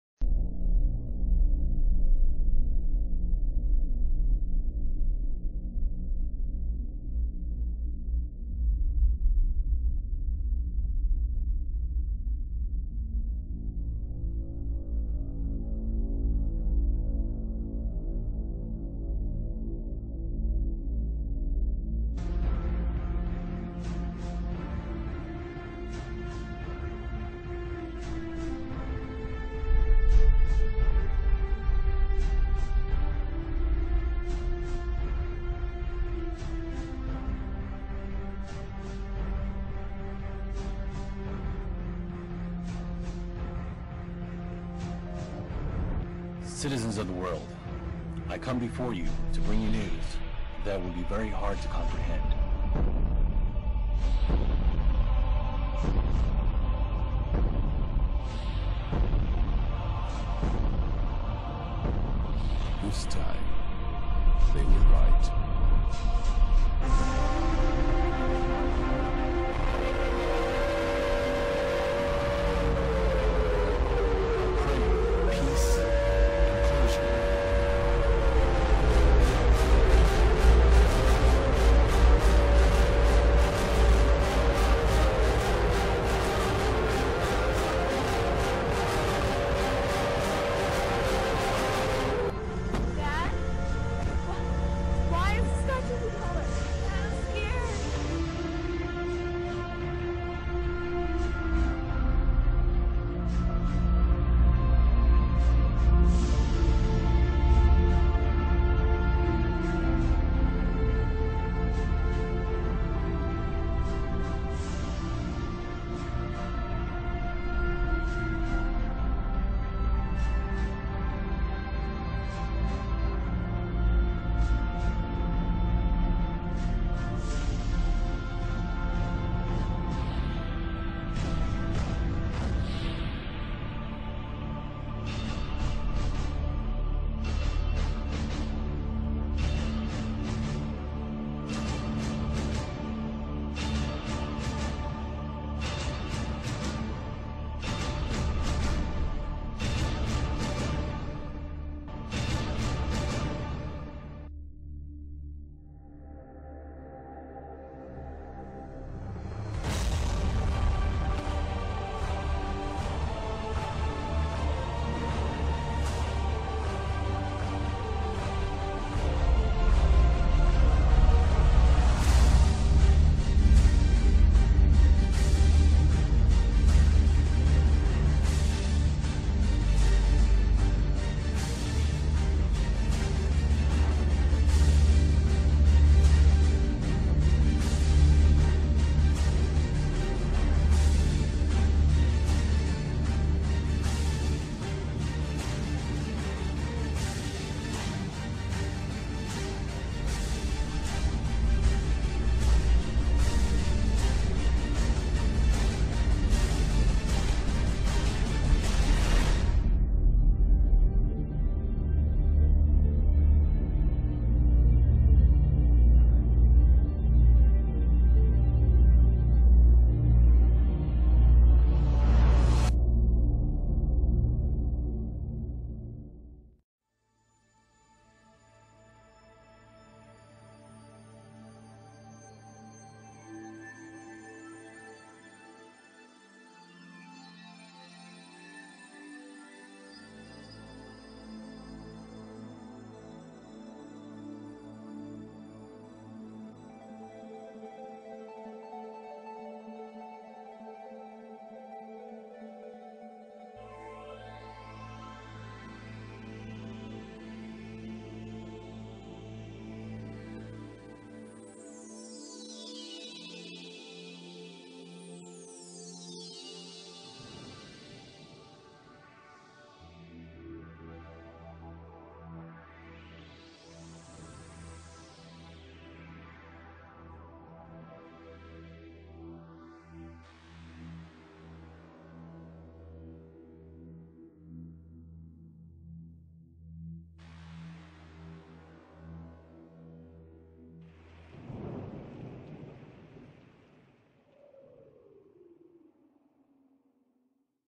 NIBIRU FACTS-Collision of the Planets,(Nibiru-World) Big LİE..
Collision of the Planets,(Nibiru-World) Big LİE..(Accurate Information)
Nibiru 3600 Year Elliptical Orbit Around The Sun(Solar System)
No Panic,No Planet Collision...